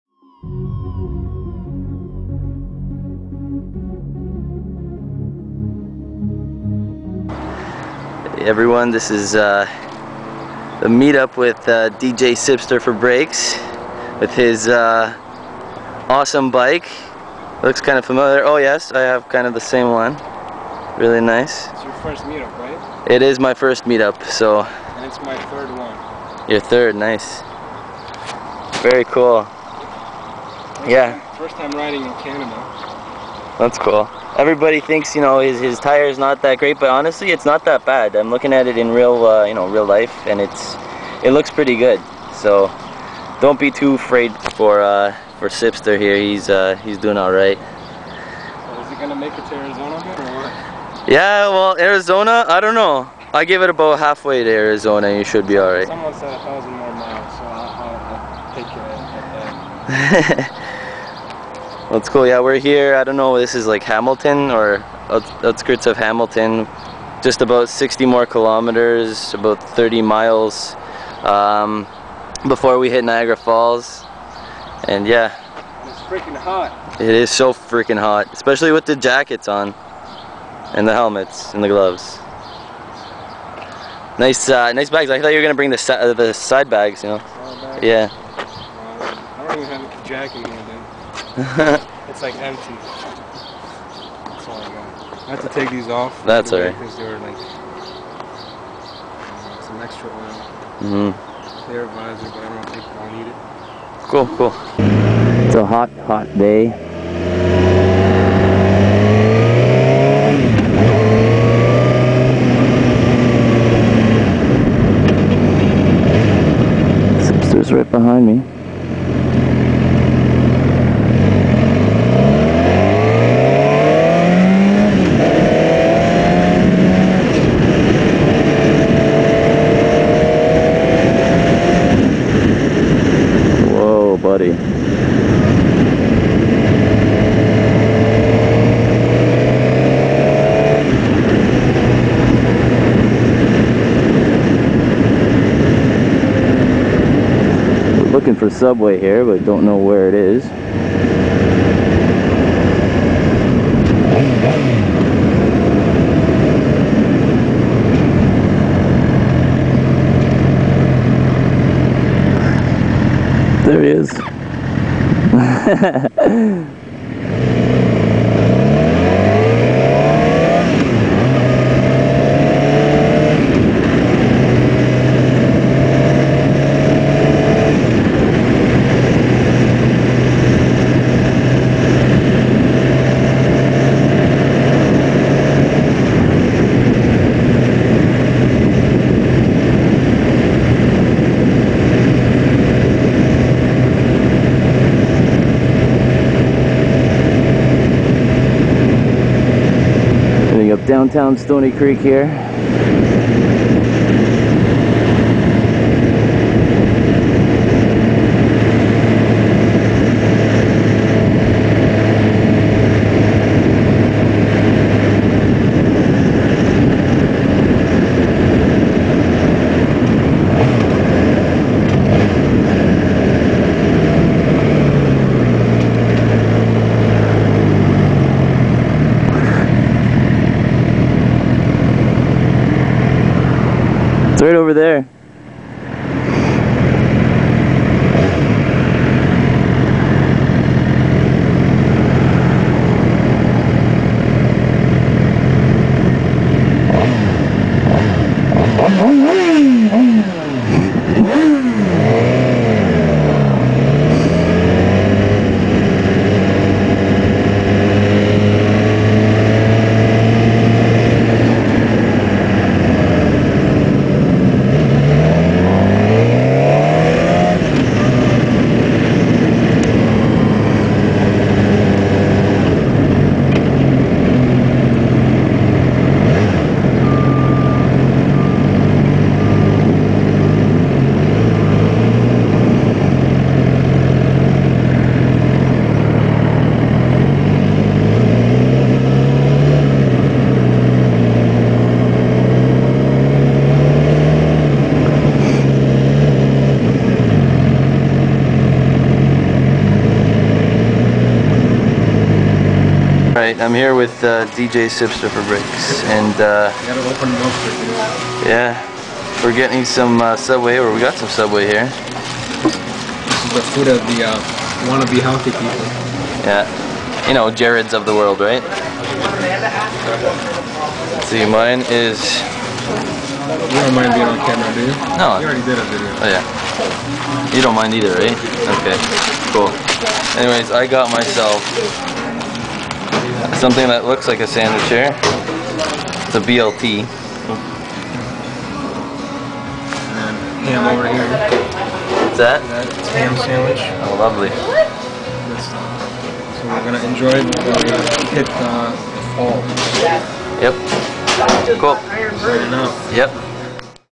Ride to Niagara Falls with DJCipster4Breaks - Part 1
My first motorcycle vlog meetup with none other than DJCipster himself!
A cool guy, a great rider, and a new friendship.
I meet with Cip and end up getting some Subway with him :)